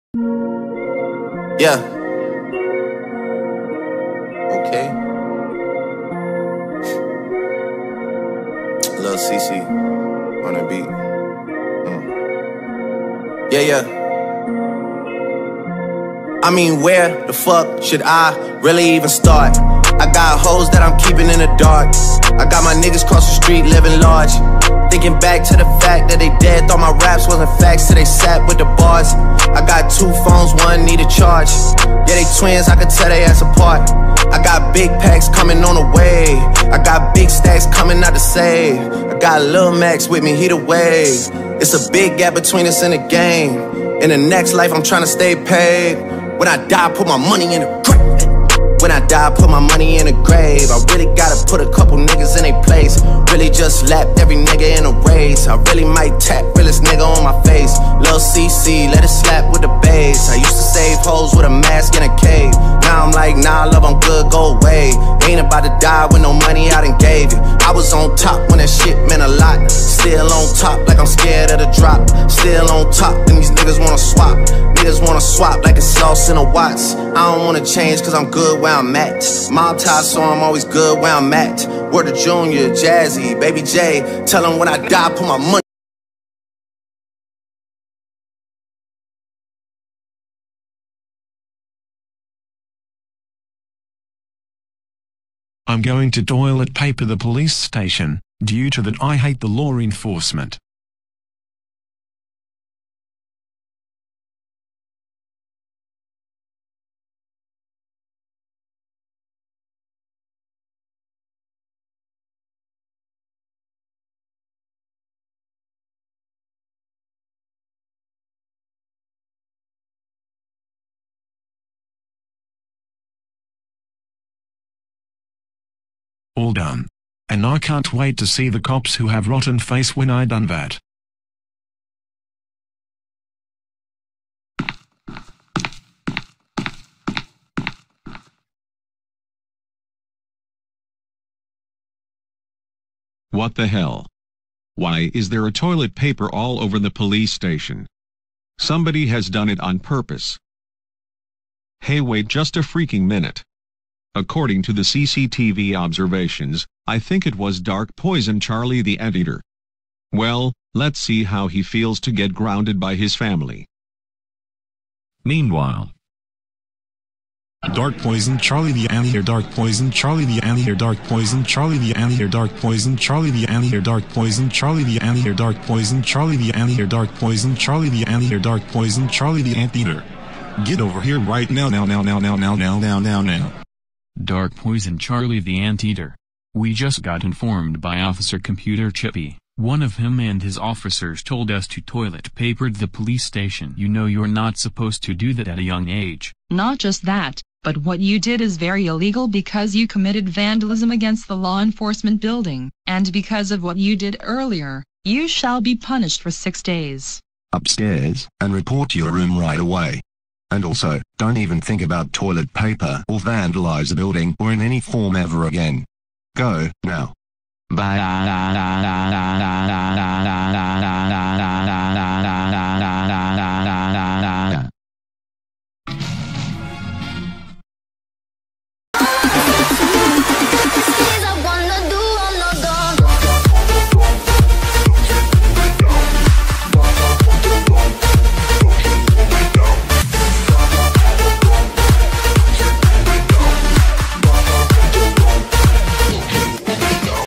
Dark Poison Charlie The Anteater Toilet Papers the Police Station/Grounded
Sorry for the lying/late request @crystalskawinski6821
anyways, It was inspired by @leonardrandomise2048 .
Don't worry, tomorrow, melanie after Dark Lava Charlie The Anteater!